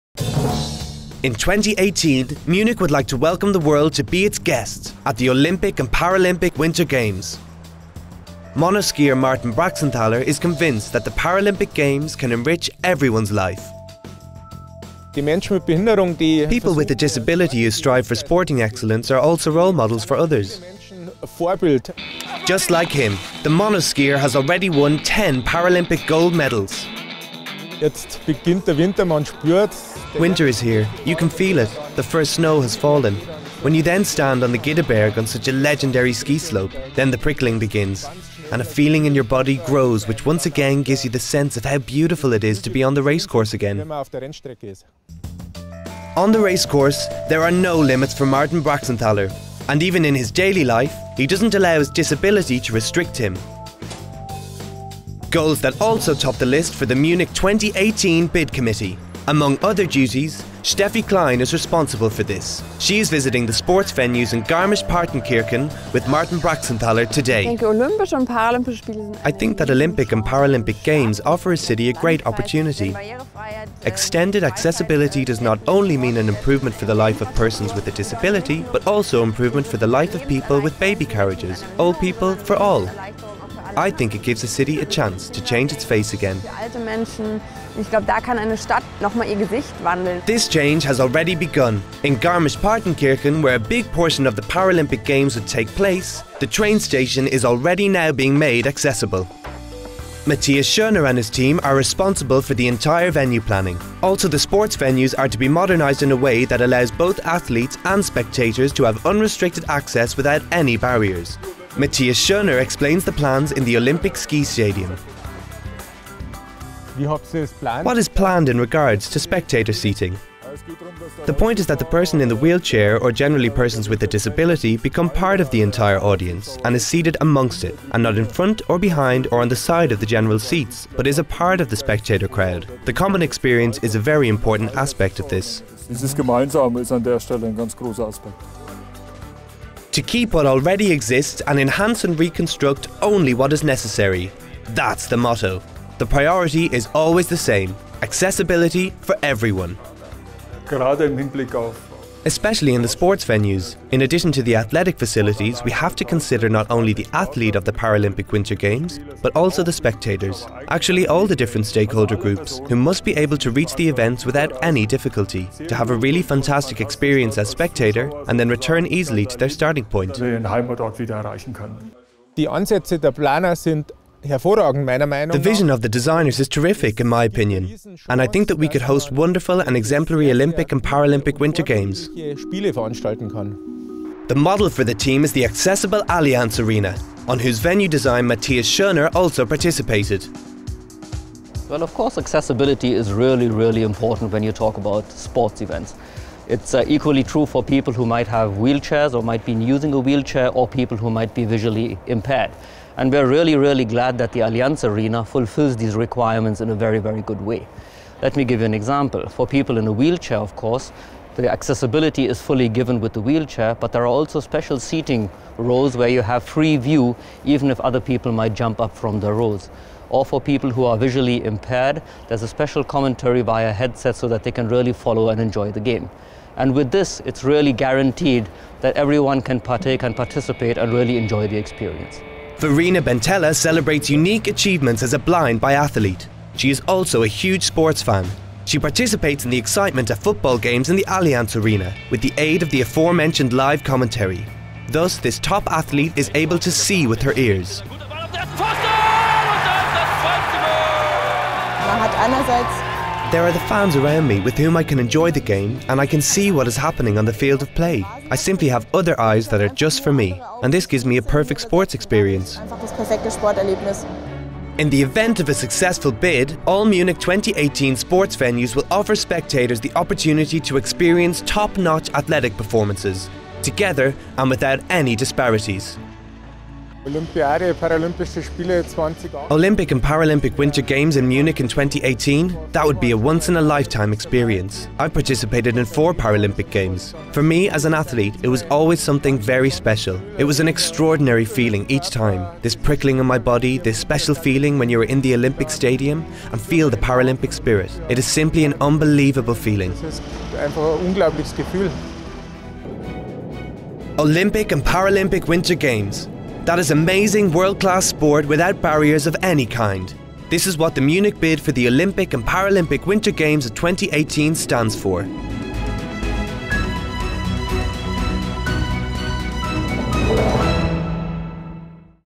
First "Living Bid Book" Published for Munich's 2018 Olympic and Paralympic Winter Games Bid "
On 11 January, the Munich 2018 Bid Committee has to submit its official Bid Book to the International Olympic Committee (IOC). The "Living Bid Book" videos help communicate essential information on some key topics of the near 400 page Bid Book to the public. The first edition, focusing on the Paralympic Winter Games, looks at the intertwined topics of accessibility and inclusion.